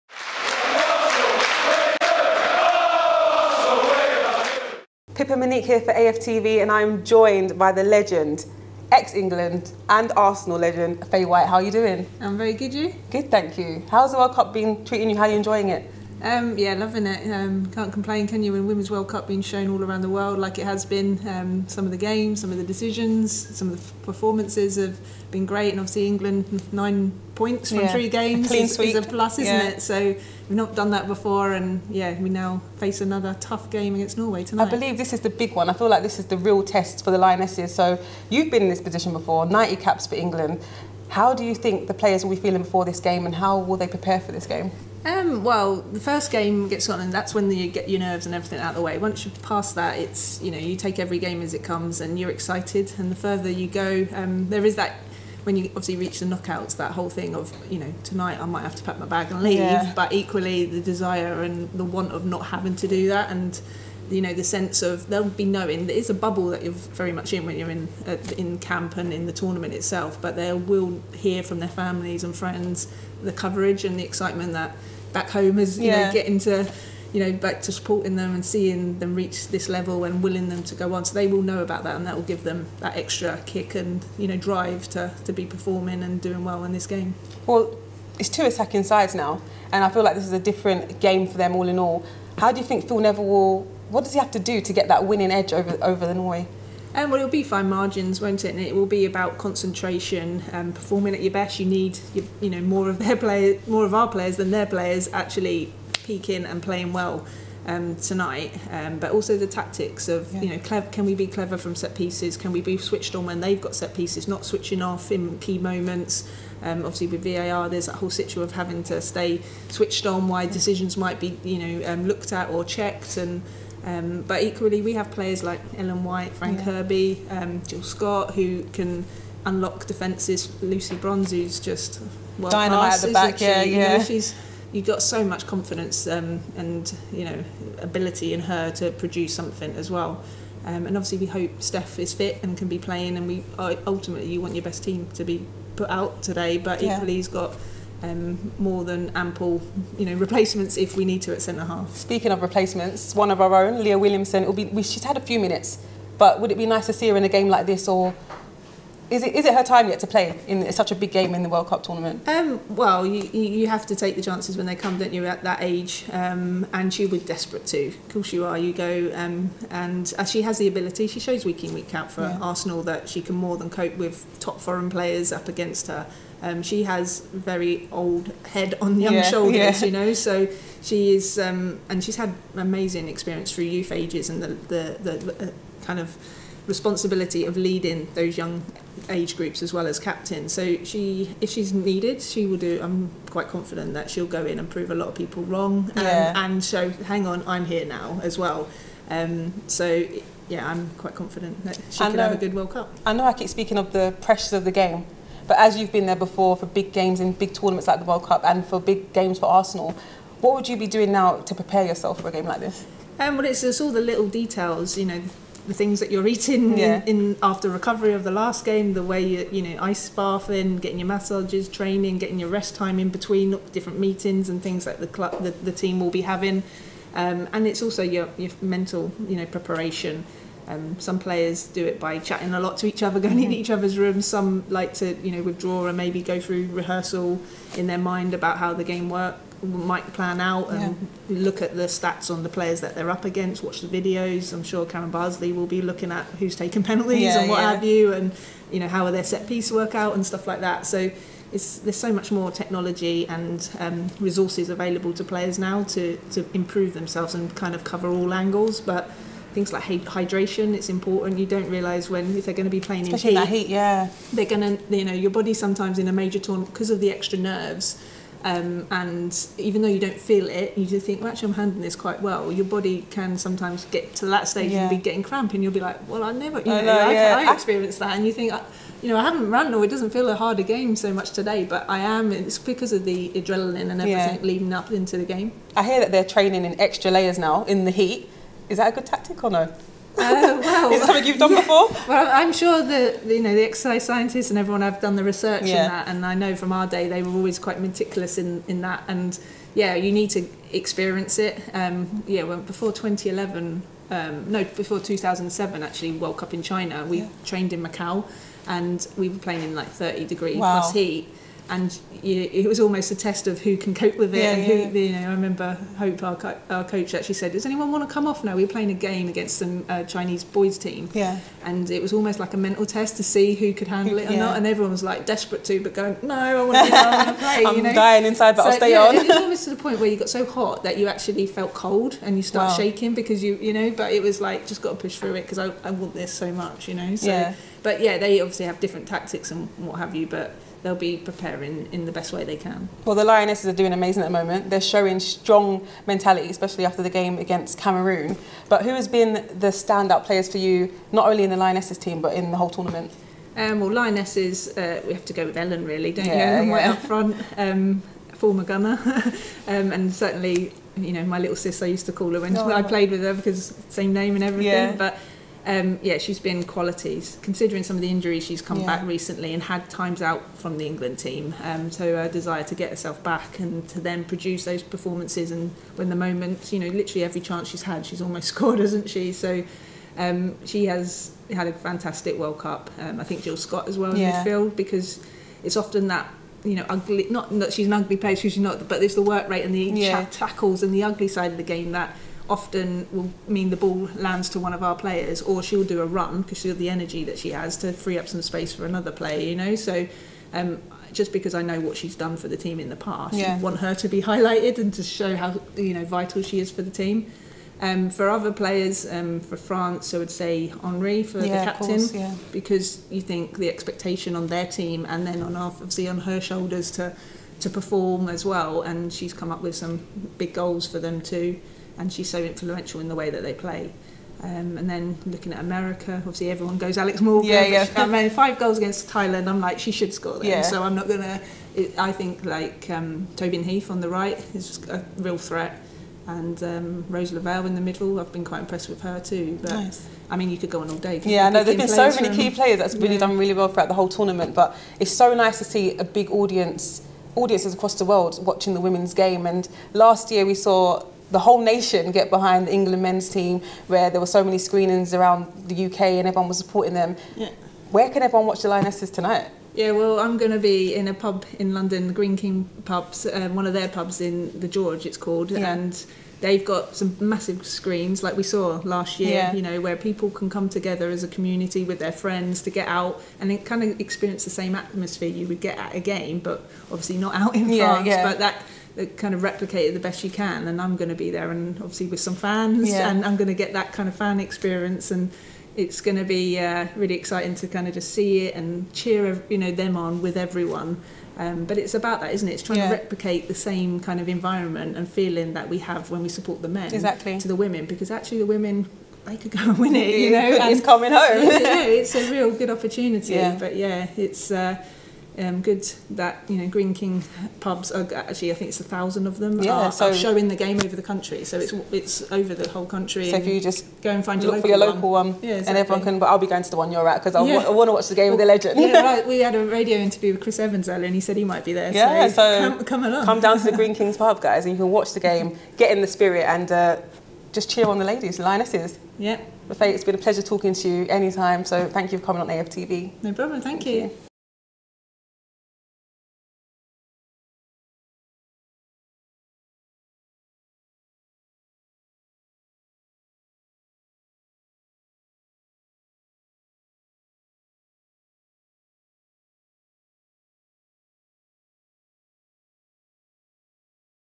The Lionesses Have Been Amazing! | England Vs Norway Preview (Women's World Cup) Ft Faye White
The Lionesses Have Been Amazing! | England Vs Norway Preview (Women's World Cup) Ft Faye White & Pippa Monique